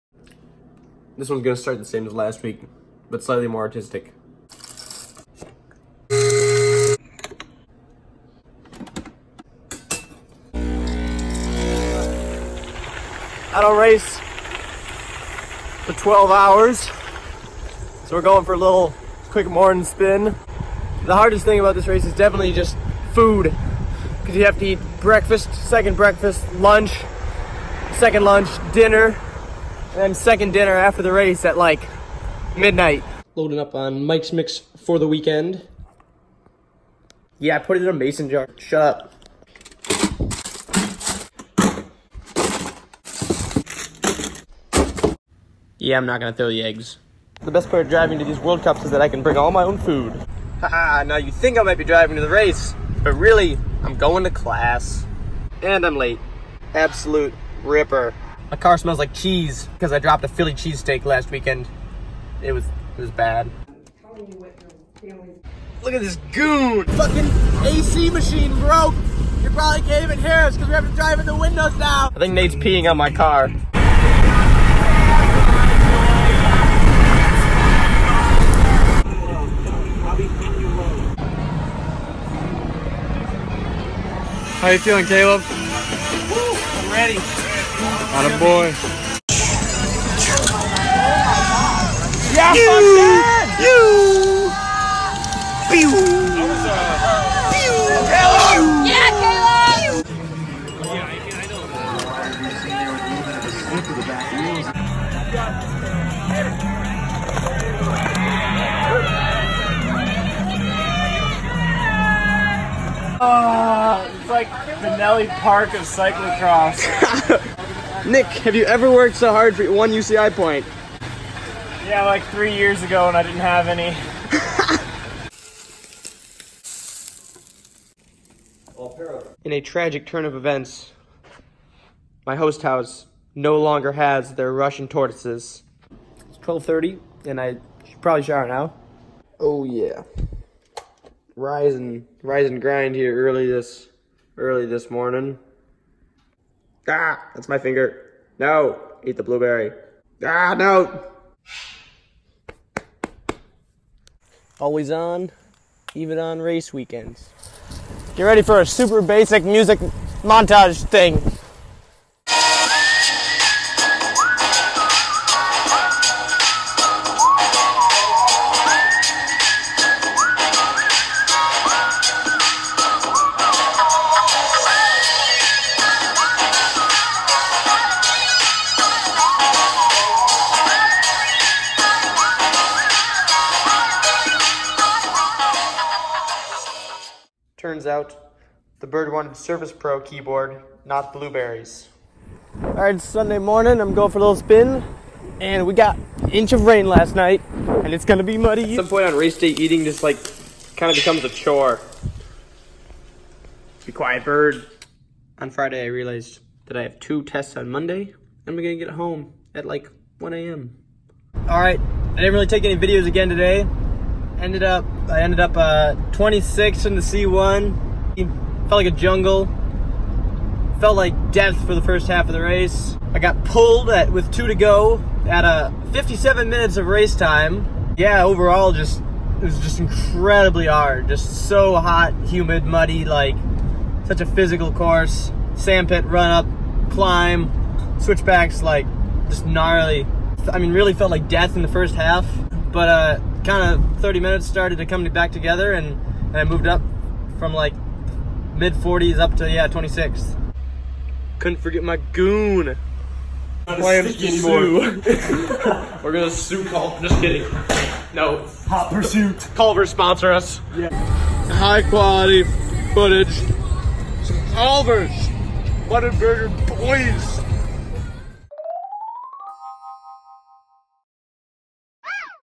Vlog 4: Jingle Cross World Cup 2019
Jingle Cross C2 and C1 in September of 2019. Plenty of mud, fun and serious racing!